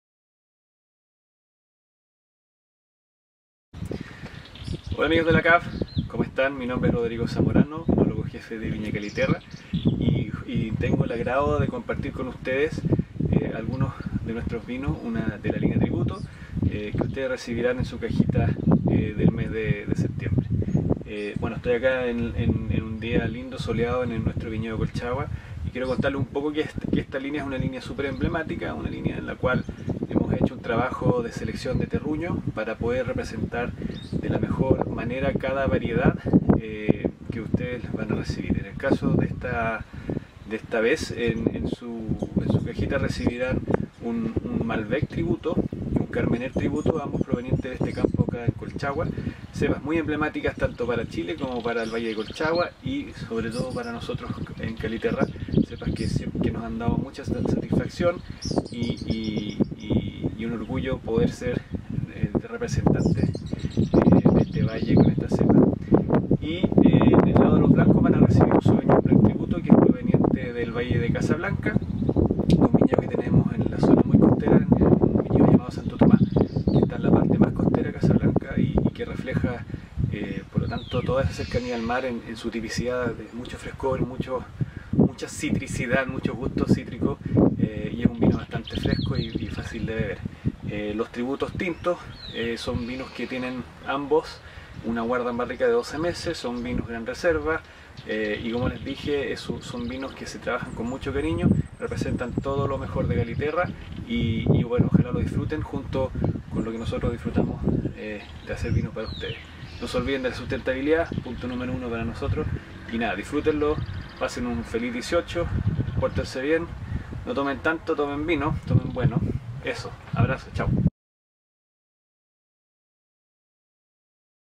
Categoría: Gran Reserva/ Vinos: Caliterra Tributo Single Vineyard
El enólogo de viña Caliterra, Rodrigo Zamorano, nos presenta un trío de vinos de la línea Tributo, una de las más clásicas de esta bodega de Colchagua, que precisamente tiene como objetivo mostrar las cualidades y ventajas de este gran valle vitivinícola.